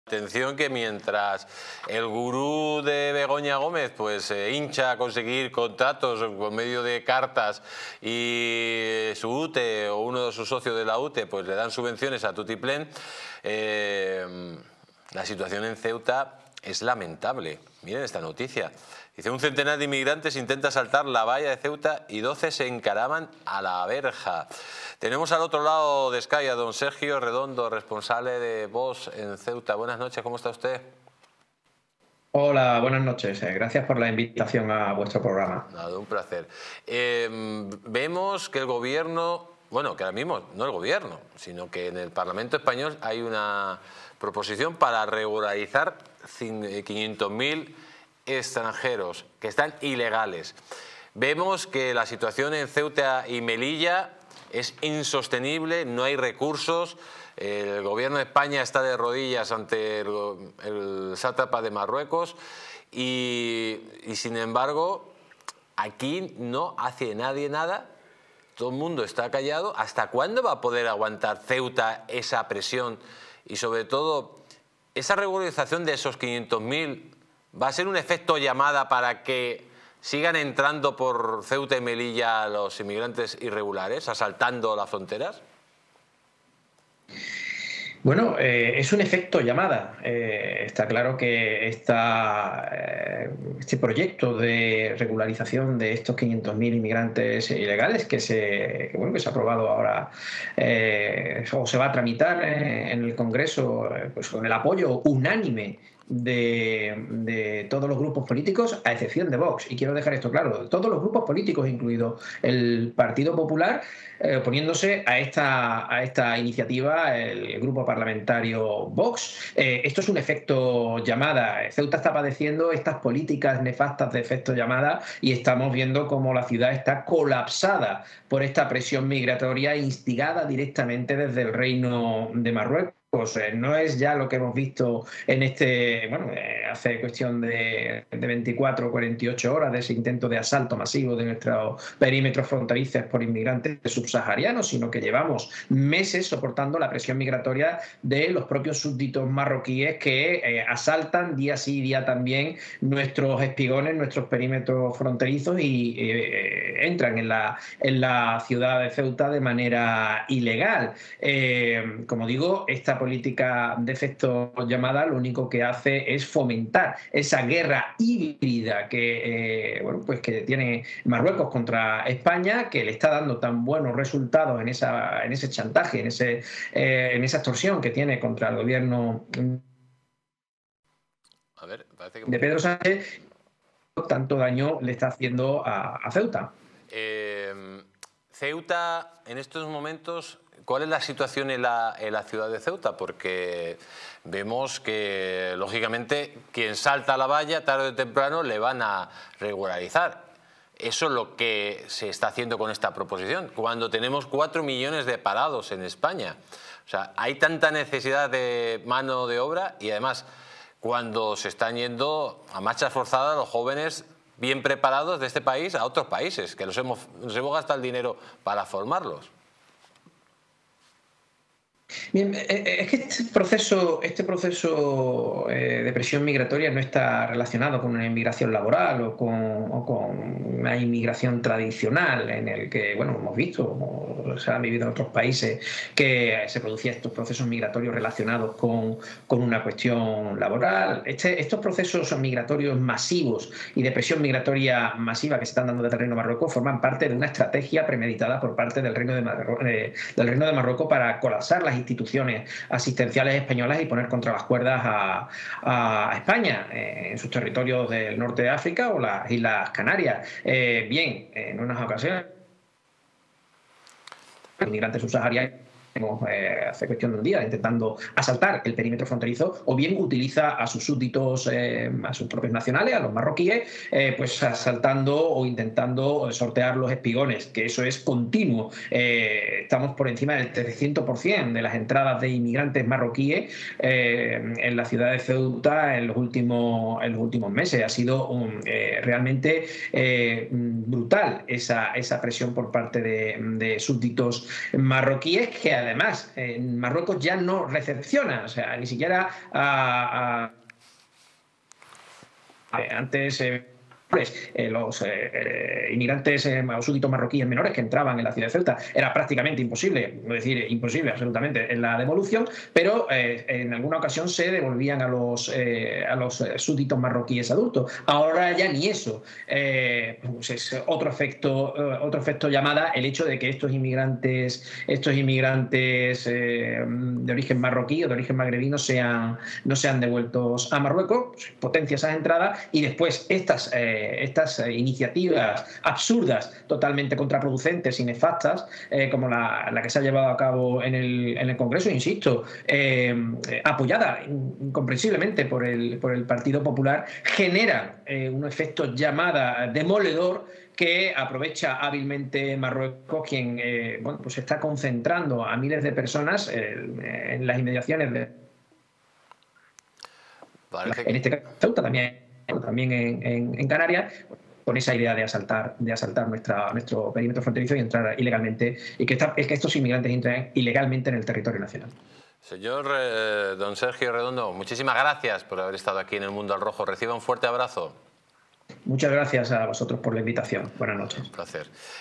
🔴SERGIO REDONDO revela los secretos del intento de asalto de inmigrantes en Ceuta🔴
En esta transmisión en vivo, SERGIO REDENDO revela todos los secretos detrás del intento de asalto de inmigrantes en Ceuta. ¡No te pierdas esta exclusiva en Distrito TV!